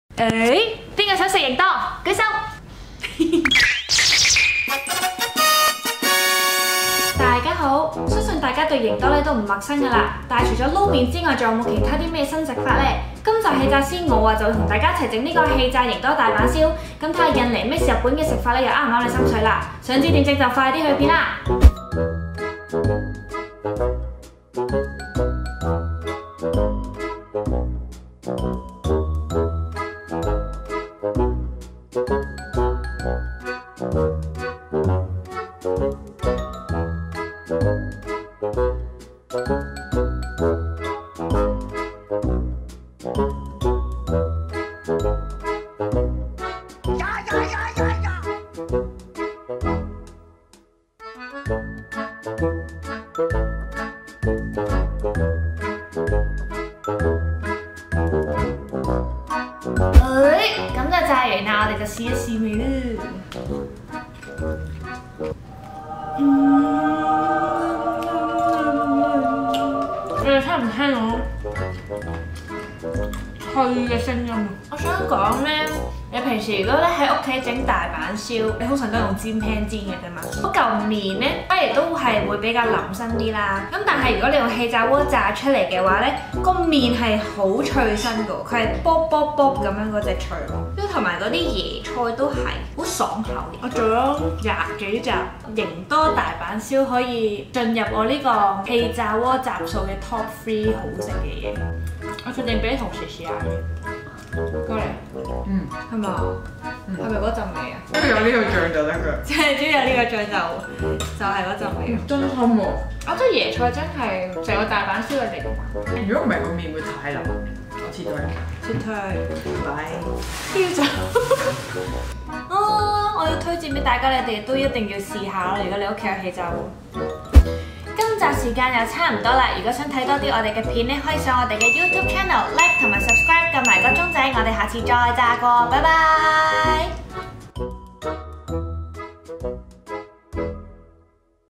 【印日Fusion小食惹味組合 DIY氣炸營多大阪燒】氣炸師
氣炸師∣  相信大家對營多麵都唔陌生，甚至成為你生活不可或缺嘅一部分，唔知除咗撈麵仲有冇其他食法呢？氣炸師今日想同大家試吓整呢個氣炸營多大阪燒，睇吓又會唔會撞到啲咩新味道啦！想知點整就快啲去片。

連結：

Too Full For Food，超過96% 真．香港讀者一直收睇！